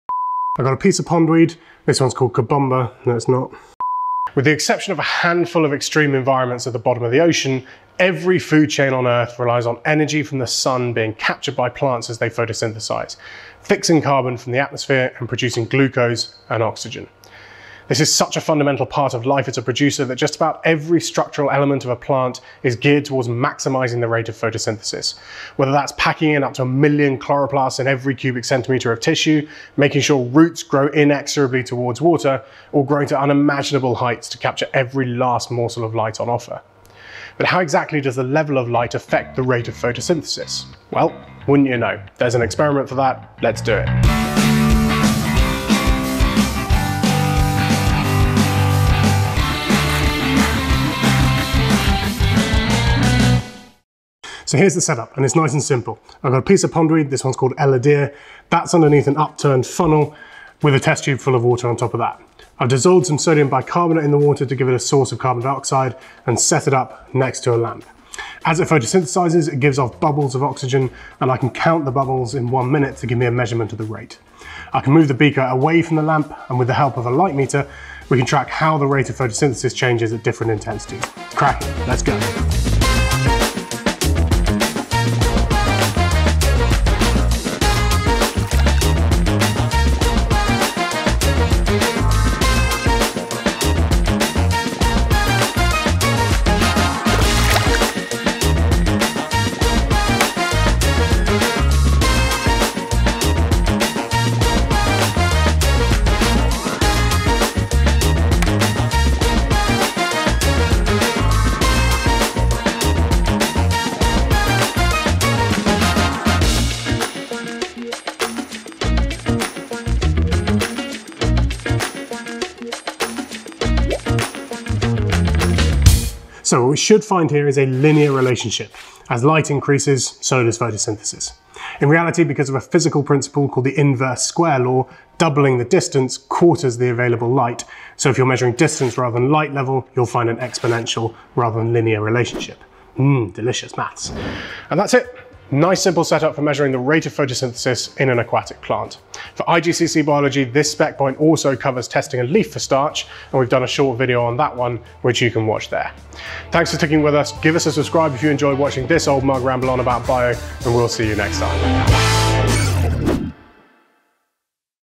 GCSE BIOLOGY: Bubbling pondweed! (Rate of photosynthesis practical)
Measuring the rate of photosynthesis by bubbling plants underwater is an absolute classic - here's a quick intro to what's going on.

Worth a note: Of course, as light levels get higher and higher, other limiting factors come into play (temperature, carbon dioxide concentration, that sort of thing) and the line would plateau. You might see that if, for example, you don't add CO2 or the lab is on the chilly side.